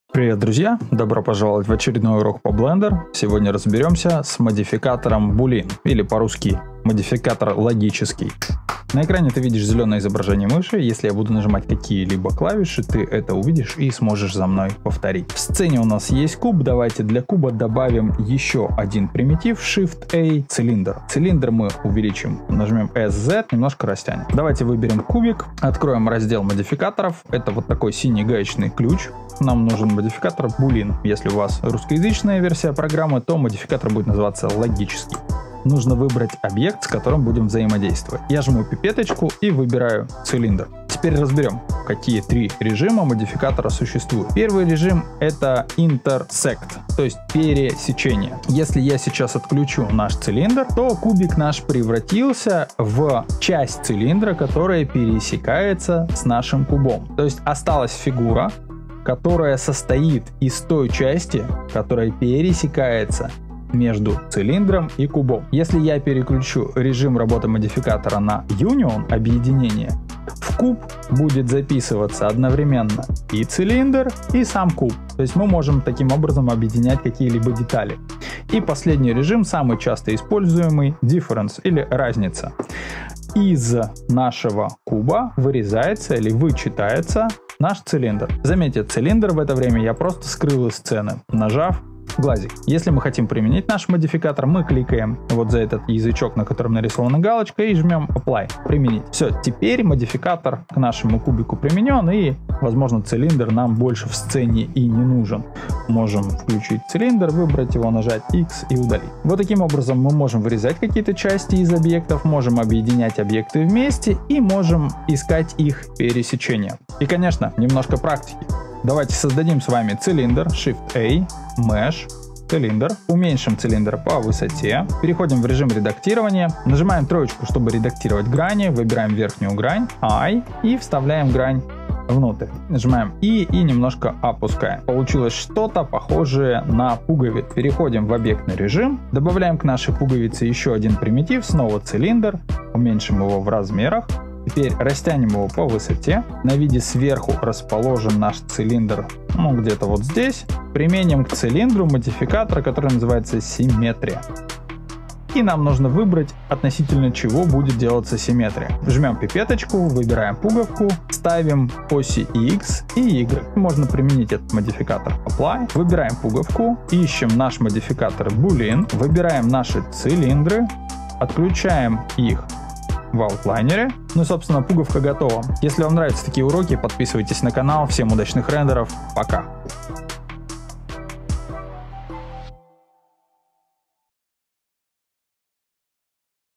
Модификатор Boolean в Blender 3.1 | Логический модификатор в Blender 3.0
Сегодня мы разберем, как работает модификатор Boolean в Blender.
Это простейший урок для новичка осваивающего 3d. Если вы новичок, то этот урок для тебя.

И как обычно работаем в Blender 3.0  или 3.1

Я пользуюсь Mac OS поэтому у меня другое название некоторых клавиш:

Аналог клавиши OS Key или Comand  - на windows это клавиша Ctrl
Аналог клавиши Option - на windows это клавиша Alt
Клавиша Return - это клавиша Enter